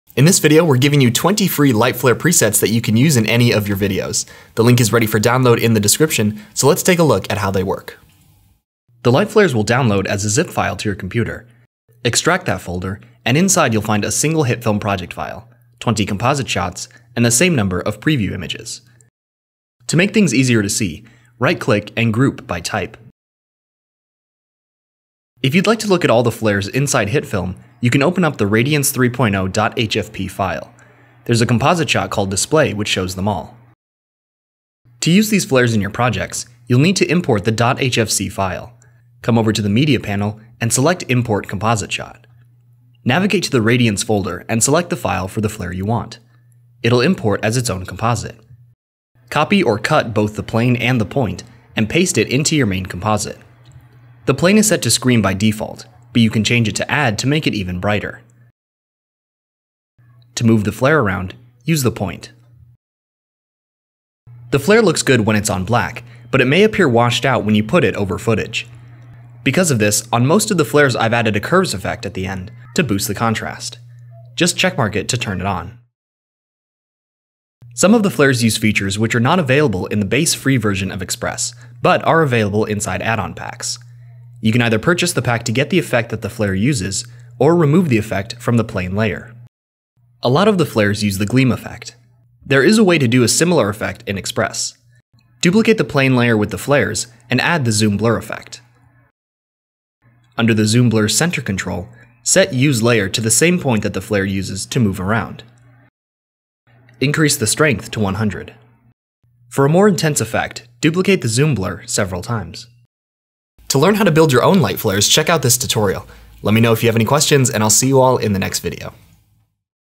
20 free light flare presets for HitFilm
We're giving away 20 new light flares to enhance your footage. We want to see what you create with these flares so be sure to send your best work to us at